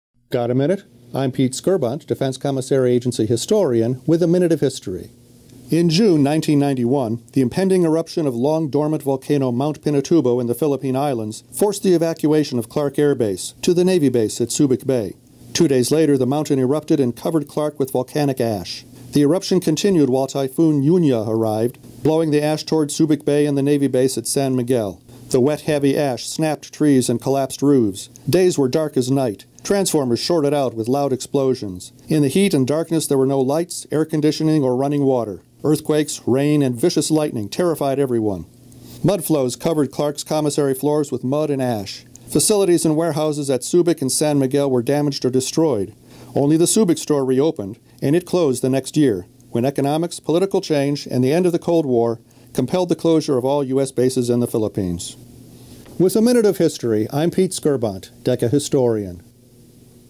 Minute of History: Mt Pinatubo
Situated in the Philippines along the Pacific's “Ring of Fire”, Clark Air Base and Naval Base Subic Bay fell victim to one of the most notable volcanic eruptions of the 20th Century and the simultaneous arrival of a major Typhoon.  The entire region felt the devastating effects, and the Commissaries were no exception. Dr. Pete Skirbunt walks us through the catastrophic events that took place 25 years ago this month.